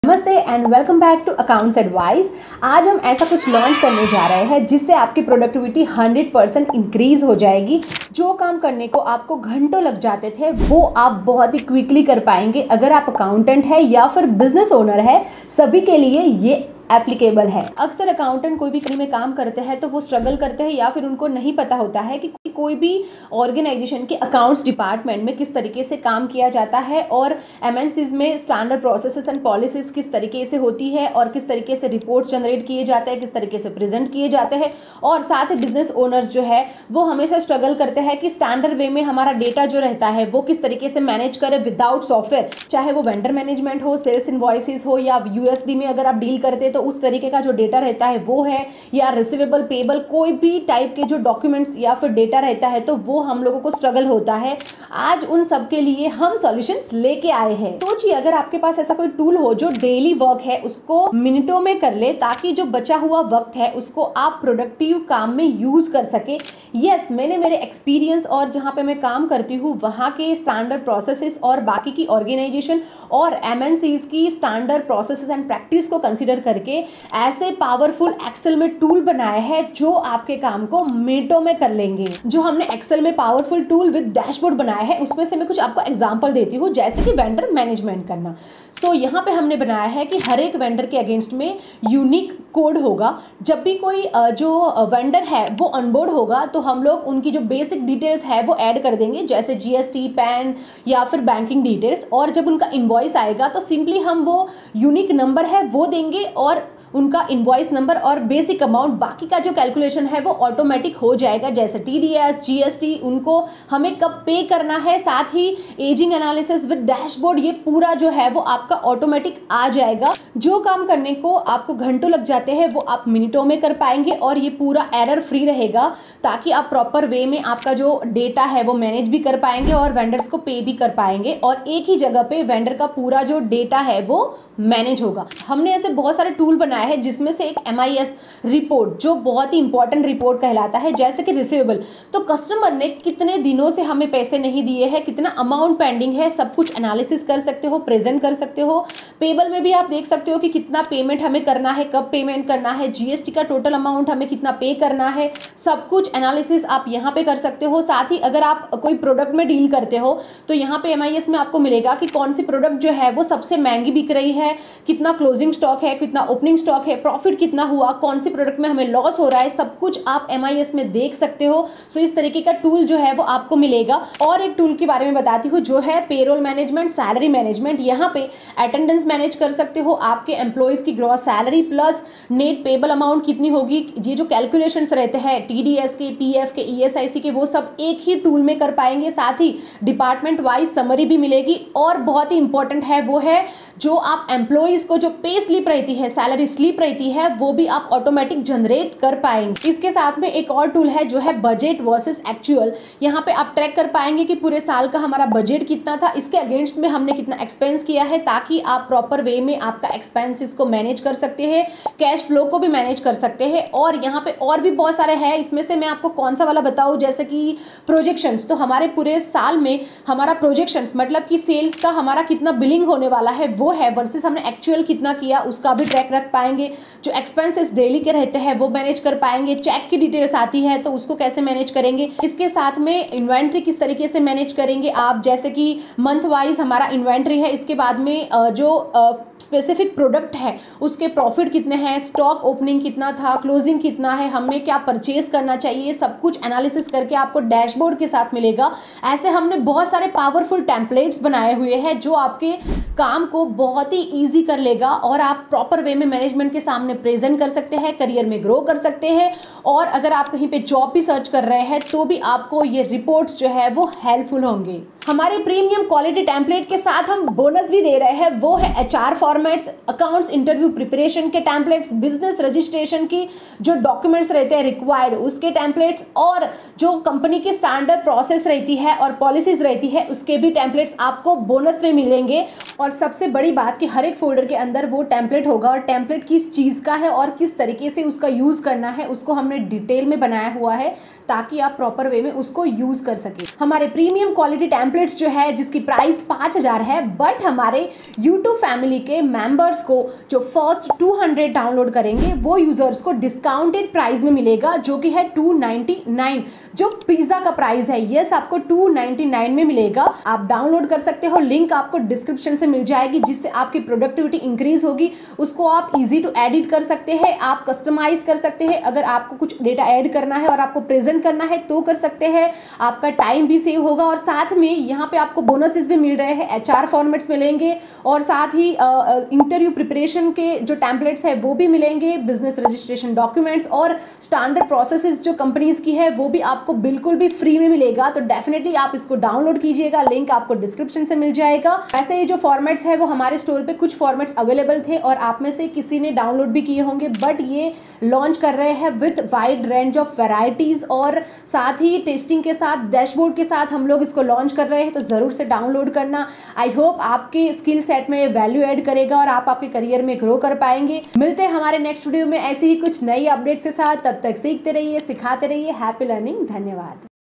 Premium Bundle Excel Tools Launch with Dashboard for Accountants & Business Owners
(Once you complete the payment , you will receive download link in the email)
Premium Excel Tools:-
We are excited to introduce our Premium Excel Tools with advanced dashboards, designed specifically for accountants and business owners. This comprehensive suite of over 50+ high-quality Excel templates, along with exclusive bonuses, will revolutionize how you manage your finances, from petty cash to profit and loss statements.
• MIS report with dashboard
• Salary Statement - Auto Calculate
• Vendor Master and Invoice Management Template with Dashboard
• Sales Invoicing Tracker with Dashboard
• Dynamic Sales Performance Dashboard Template
• Accounts Receivables Tracker for Efficient Accounting in Excel
• Accounts Payable Management System
• UDS Billing management
• Trial Balance Template
• Balance Sheet Template for Financial Insight
• Automated Profit and Loss Statement
• Cash Flow Statement Template
• Budget Control Sheet
• Inventory management
• Yearly Projections management
• ROI Per Employee Calculator
• Sales Pipeline Tracker
• Income, Expenses,Profit & Loss Template
• Track Employee Attrition Efficiently
• Gratuity Calculator
• Cheque Book Register Template!
• Product _Sales Revenue Data Analysis Template
• Inventory Management
• and many more (50+ templates)
Exclusive Bonuses:
 HR Related Formats
 Accounts Interview Preparation
 Company Standard Processes
 Business Registration Documents

Why Choose Our Premium Excel Tools?
Premium Quality Excel Templates: Professionally designed for optimal performance and accuracy.
Increased Productivity: Save time and effort with our efficient tools.
Easy to Edit & Customize: Tailor the templates to meet your specific needs effortlessly.
Wide Variety of Templates: Covering all aspects of financial management.
Time-Saving Solutions: Focus on what matters most by reducing repetitive tasks.

Boost Your Efficiency and Career Growth!
Our Premium Excel Tools are designed to help you achieve greater efficiency and accuracy in your financial management.

Timing:
0:00  Introduction Launch
1:27  Tool Examples
4:25  Excel tools Launch with Bonuses
4:54  Prices of Excel tools
Download Excel formats:-
Store Link- Accounting solutions 4U

My Recommended Products [I will only share what I love💜 here]

All opinions expressed on my channel are solely my own and should not be considered as financial or professional advice.